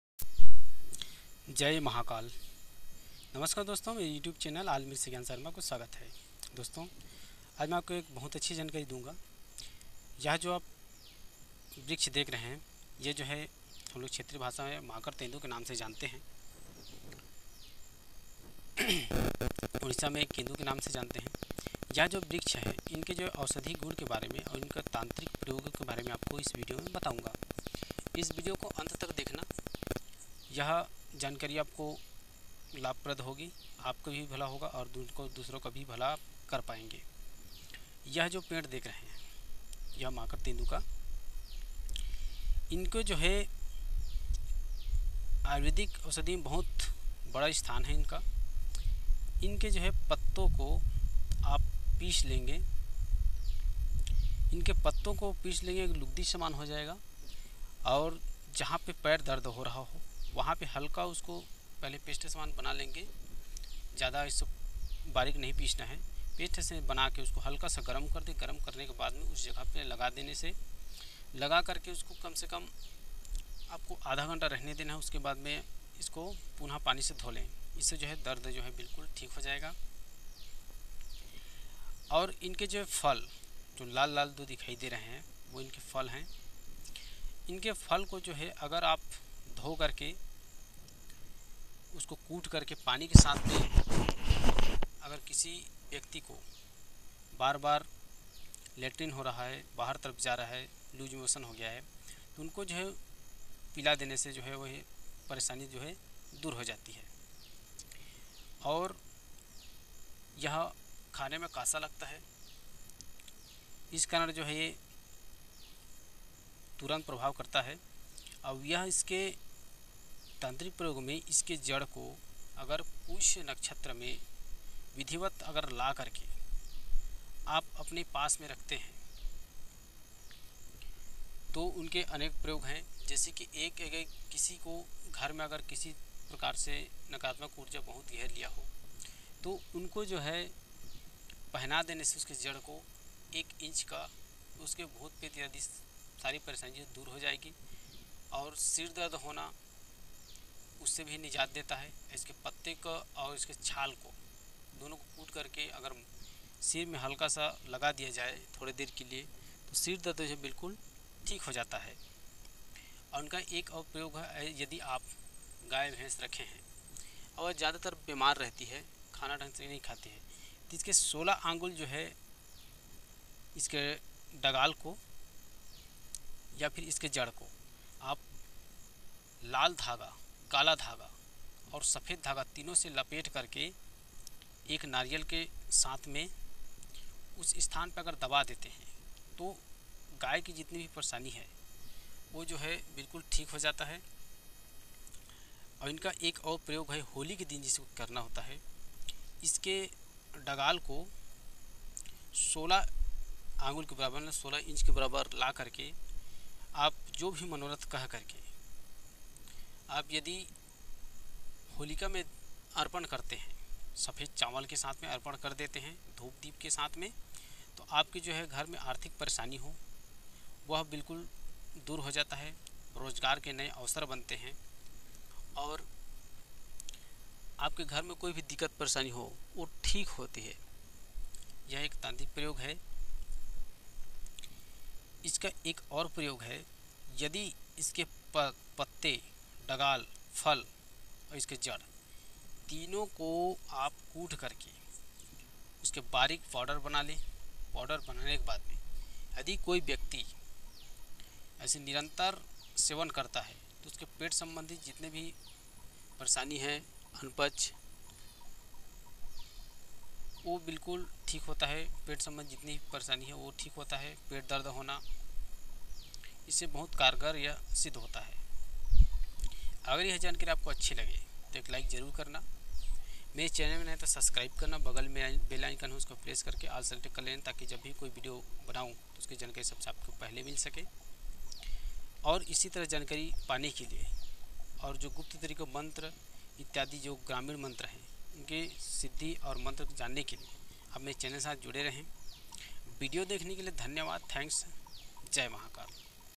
माकर तेंदु का रहस्यमयी औषधीय प्रयोग।।makar tendu
नमस्कार दोस्तों

Is video mein man Karti Doge Raste Mein aushadhi prayog ke bare mein bataya gaya hai..

अन्य वीडियो..